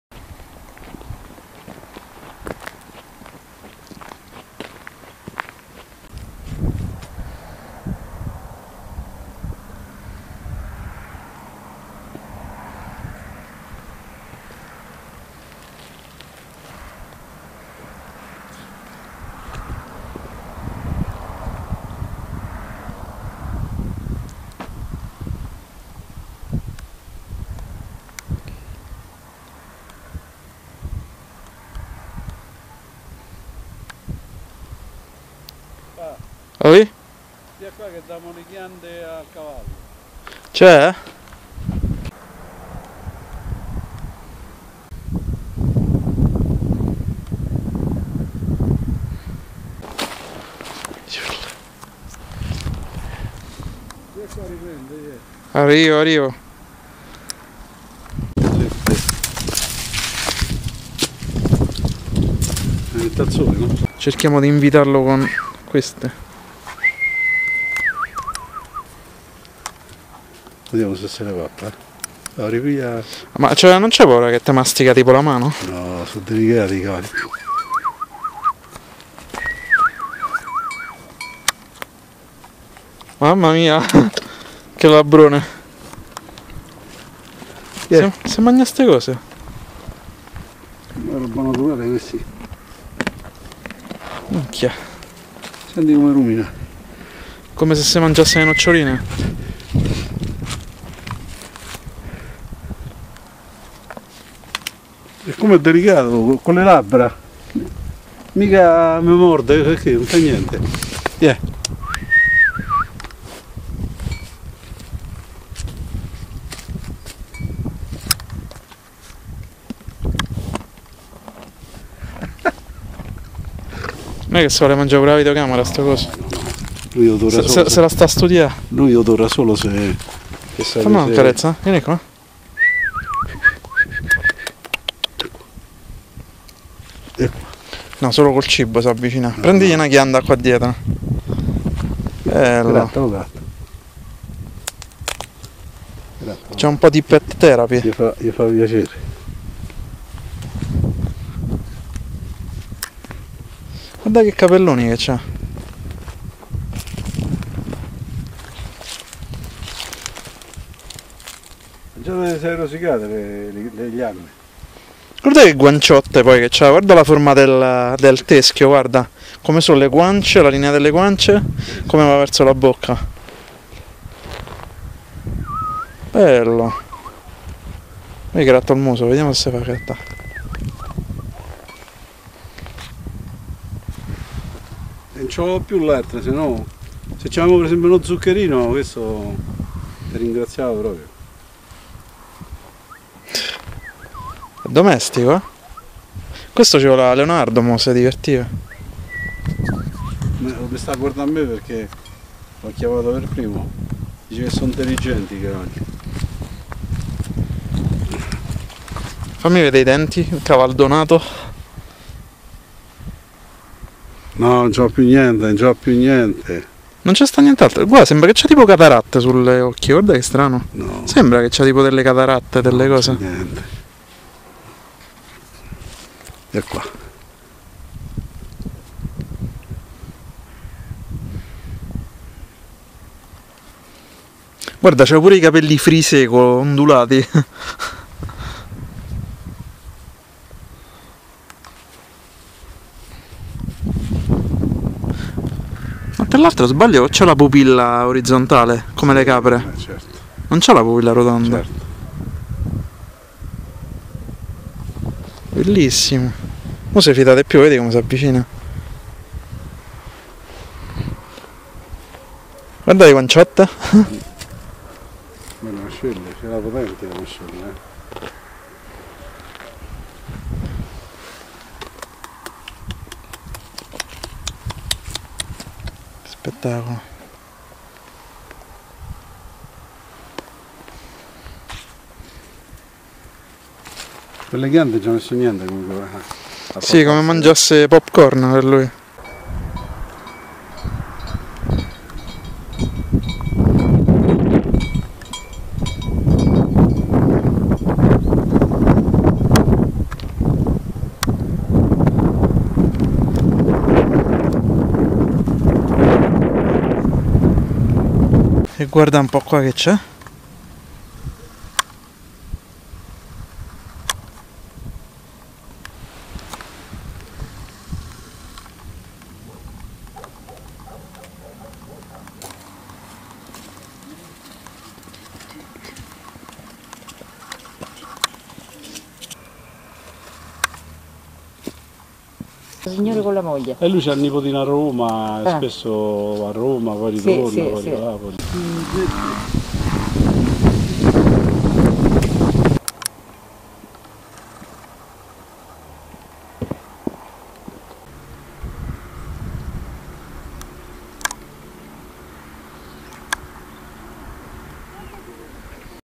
Porta Saracena in Segni
Walk at the Porta Saracena in Segni (approx. 1 hour drive from Rome), where we met a beautiful horse that we fed with acorns.